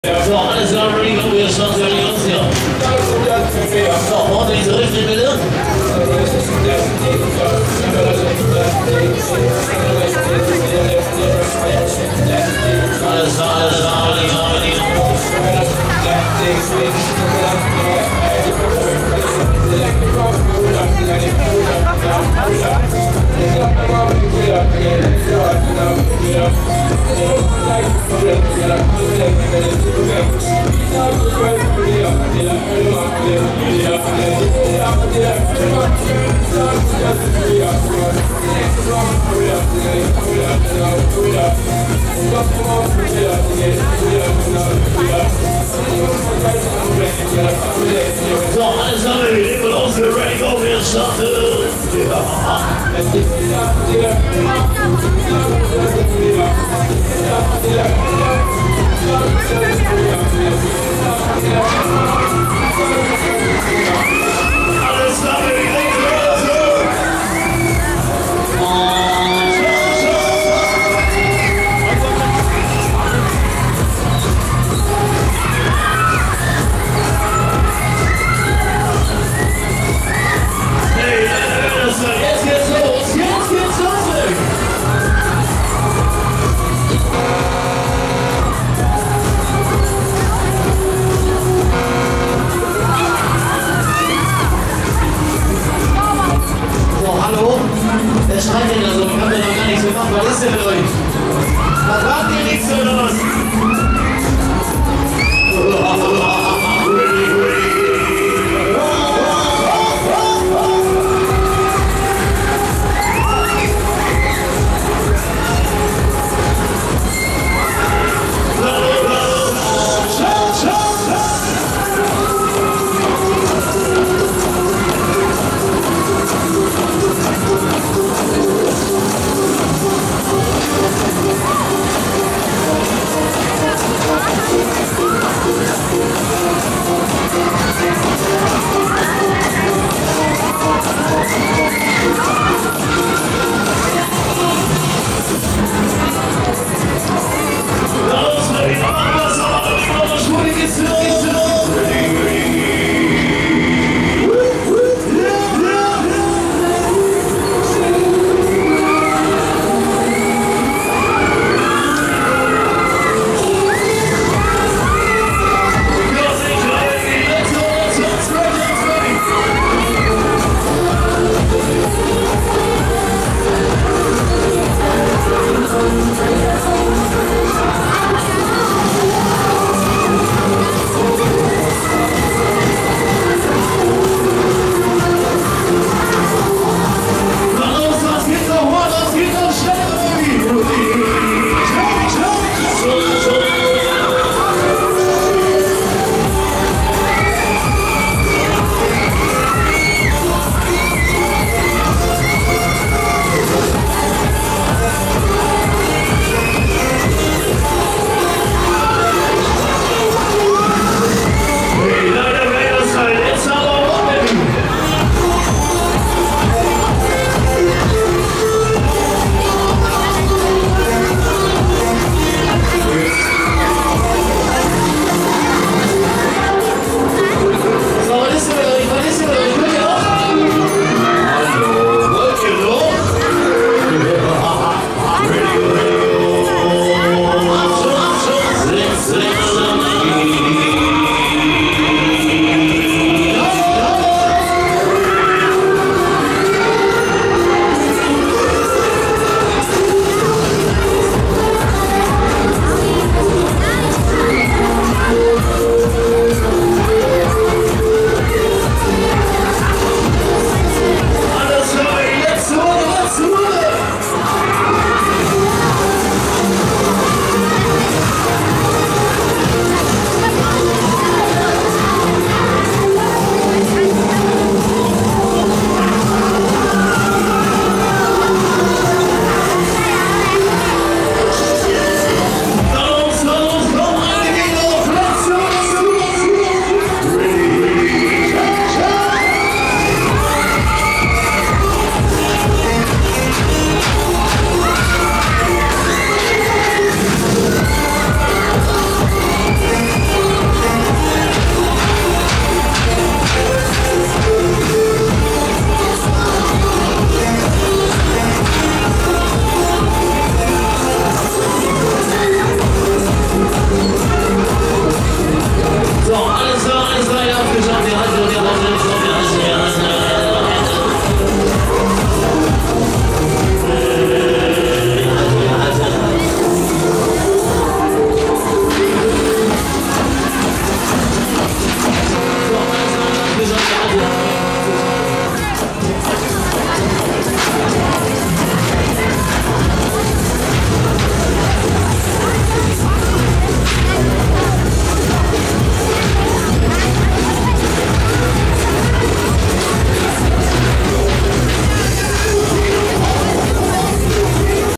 Break Dance (Bonner) - Außenansicht/Offride [Bottrop - Karnevalskimres 2017] 4
Break Dance vom Schaustellerbetrieb Bonner auf der Karnevalskirmes in Bottrop 2017.

Break Dance Marke Huss